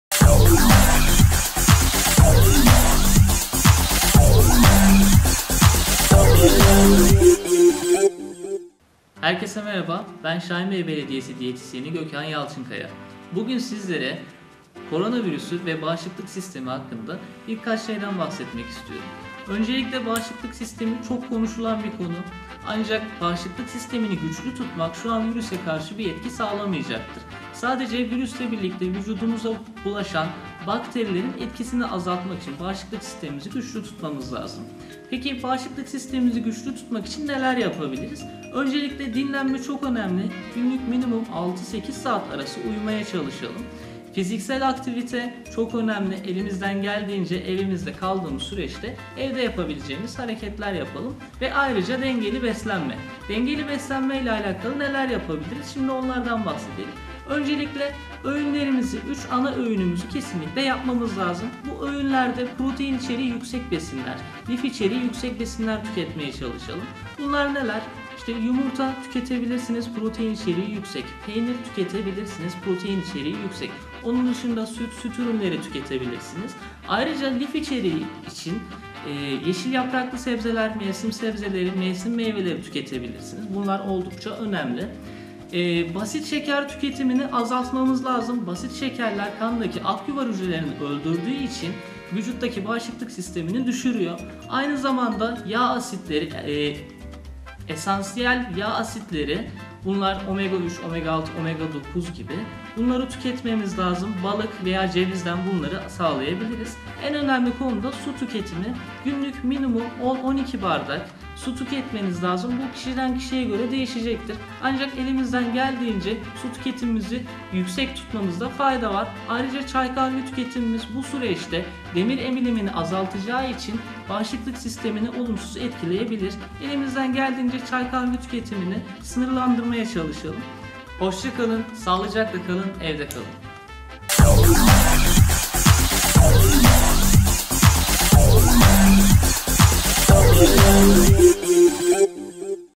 Koronaya Karşı Bağışıklık Sistemimizi Nasıl Güçlendirebiliriz
Siz Değerli Vatandaşlarımızın Virüsten Korunmasının Yolu Evde Kalmaktan, Evde Kaldığımız Süreçte İyi Hissetmenin Yolu da Sağlıklı Beslenmeden Geçiyor.